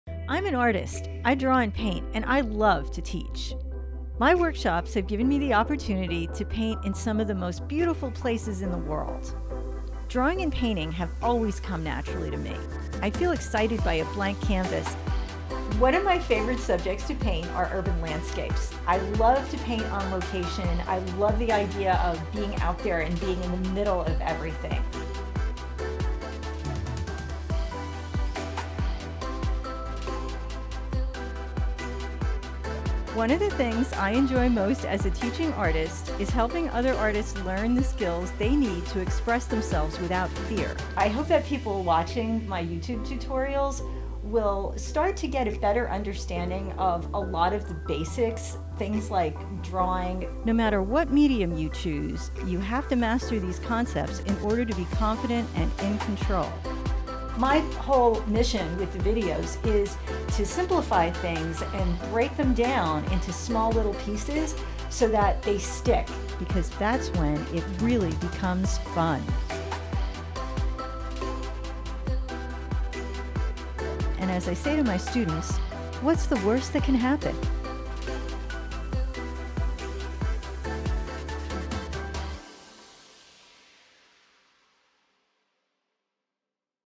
Meet Anne Kullaf
Anne Kullaf is a professional fine artist and instructor. Formally trained in graphic design and painting, she has had a highly diverse career as a designer, entrepreneur and visual artist. She has taught at the Princeton University Art Museum, Edward Hopper House, the Visual Arts Center of New Jersey, and the Center for Contemporary Art, and has led plein air painting workshops in the US and in France, Italy and Canada.